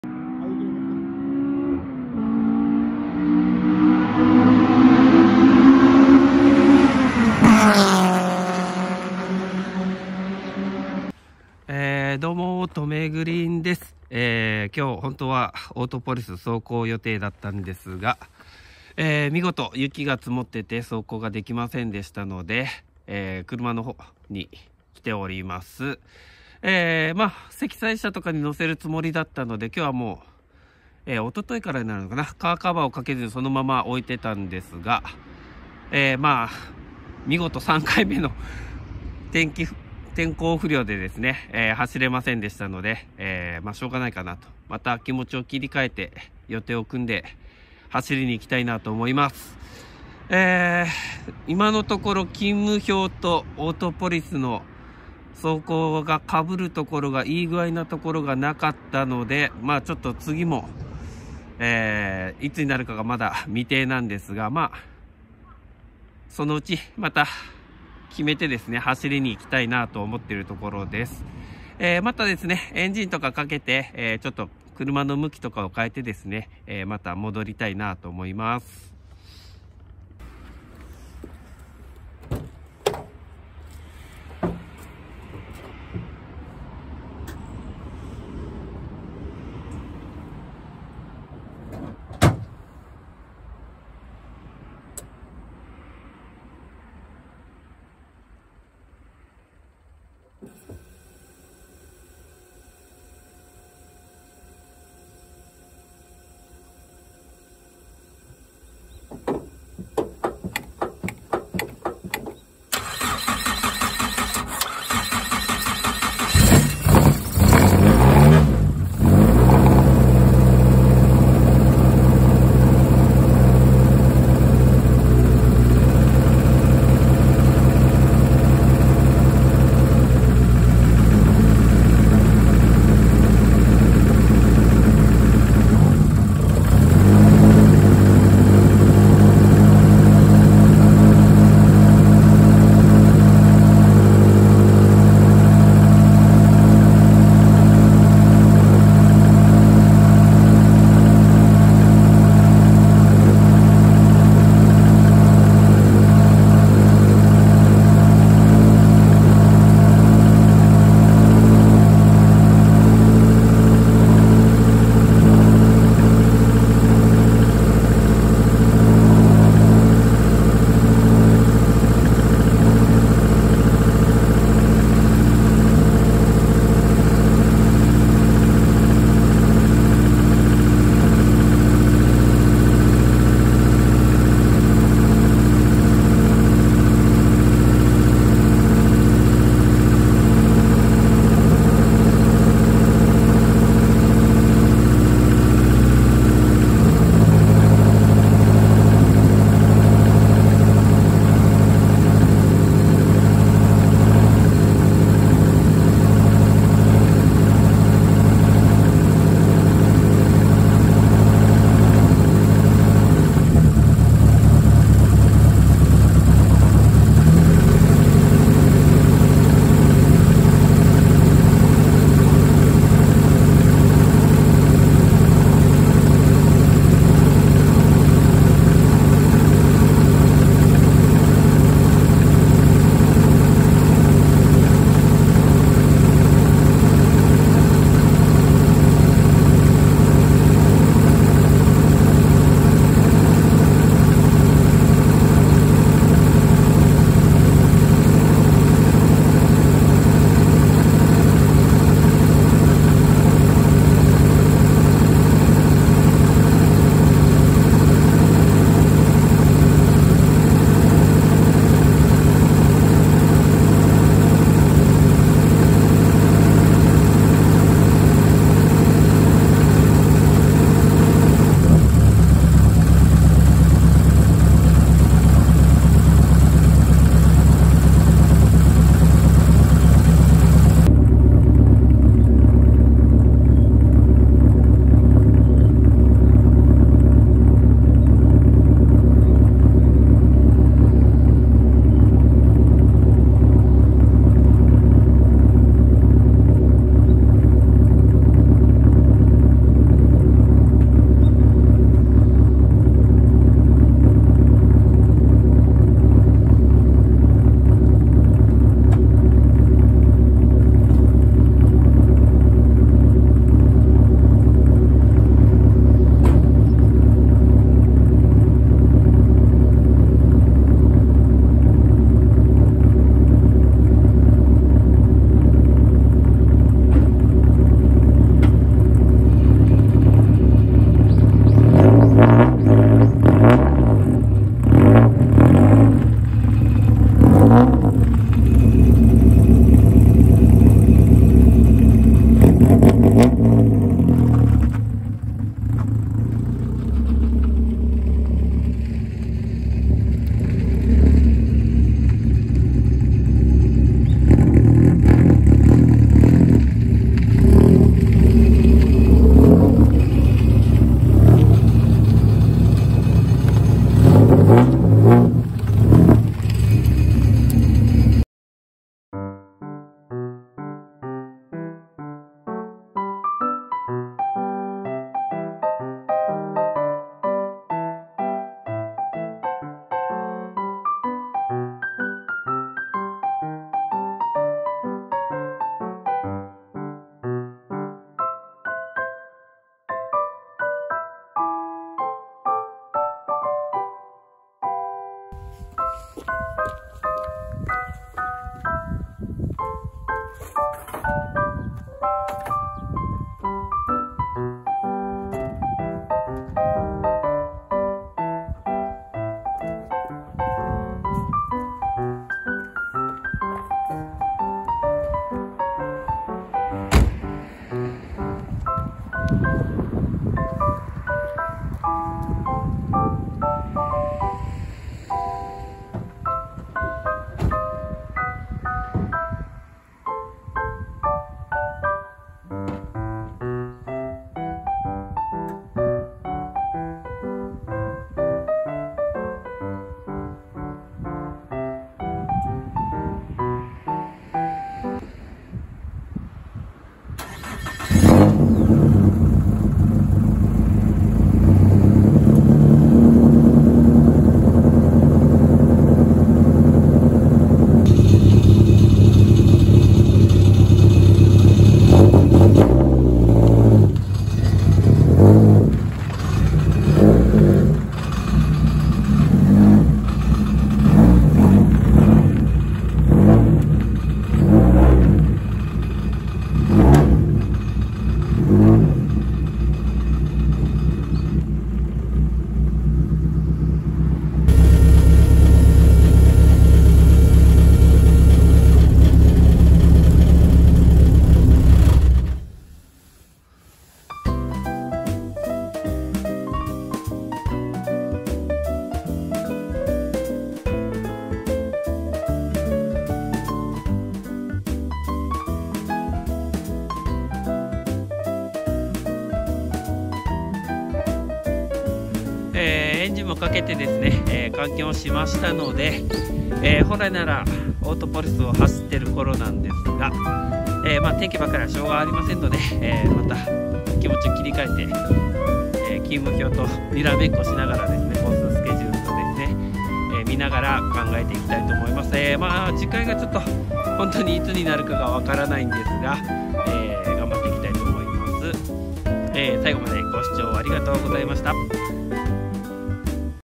積雪がなければオートポリスサーキット走行予定でした🐸💦
興味を持ち見ていただいてありがとうございます。

今日の動画は、積雪の為、オートポリスサーキットが施設クローズで走れなかったので駐車場で動かしてみた動画です🙇‍♂️

s13シルビア　NA  2.2L  4連スロットル仕様です。

オートポリスサーキットのグリップ走行復活しました🙇‍♂️

ナンバー無し・グリップ走行・タイムアタック車両です。

トランク内をカットしてフラット化しています。

純正タンクから20Lの安全タンクに変更しています。

ドンガラ室内、ワンオフ溶接留めロールバー🙇‍♂️

ECUは、LINKモンスーンを使用。

ECU変更に合わせ純正ハーネスを撤去してハーネスもワンオフしてもらっています。

ドライカーボンルーフ等で軽量化。

ウォーターポンプをEWP化。パワステは、MR2の油圧式電動パワステに変更しています。

今回は水冷オイルクーラーを使用しています。

実測係数1.0で193馬力〜係数1.15で約220馬力でした。

オートポリスは、音量規制がないので直管仕様にしています。